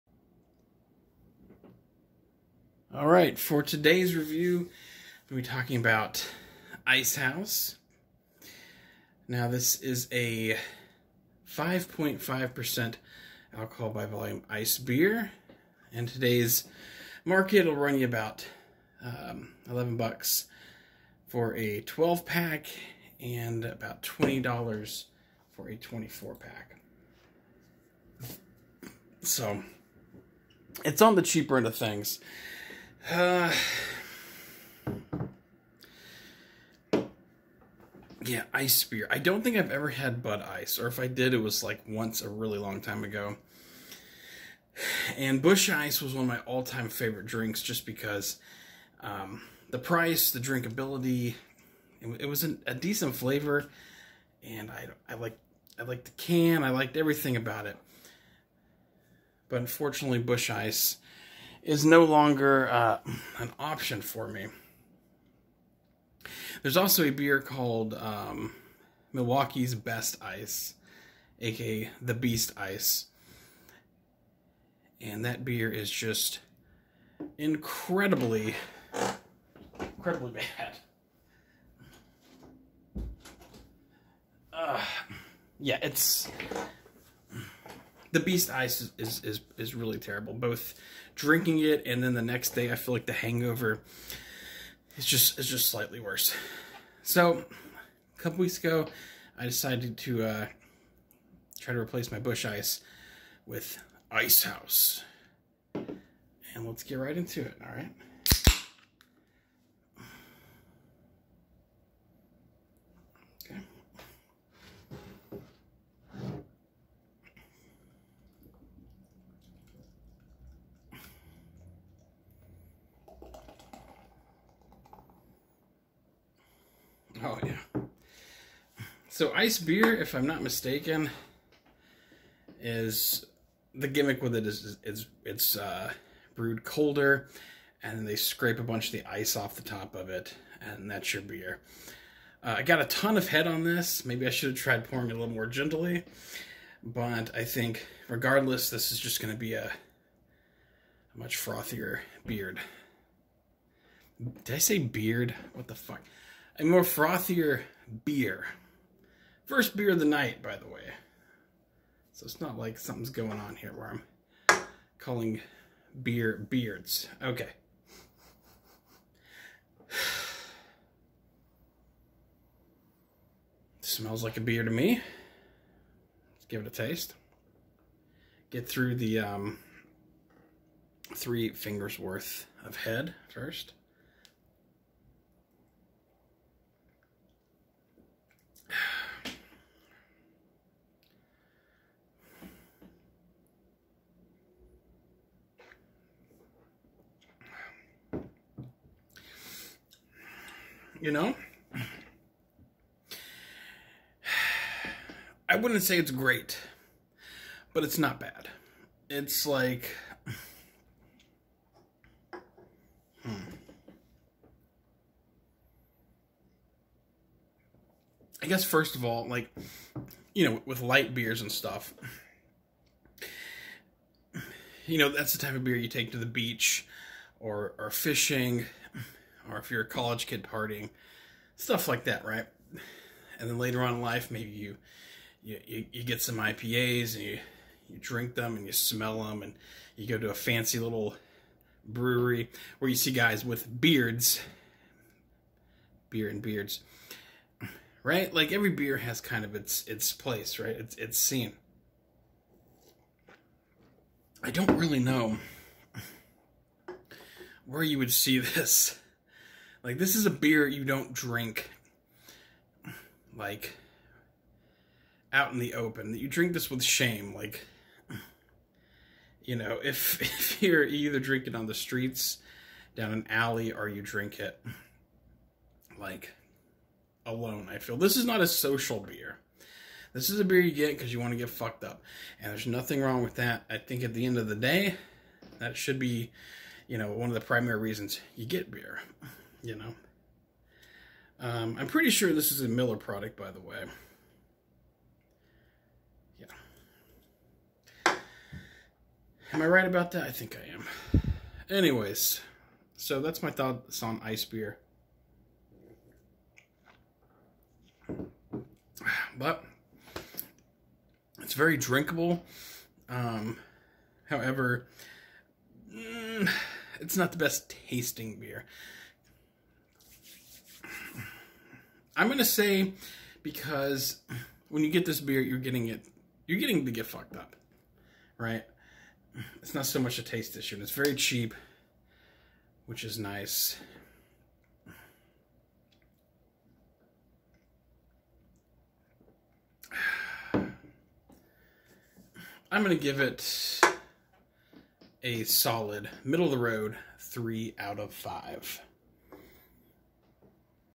Beer Review 5 | Icehouse
The best Ice Beer on the market.

Style: Ice Lager | Tap/Can/Bottle: Can | Abv: 5.5% | Rating: 5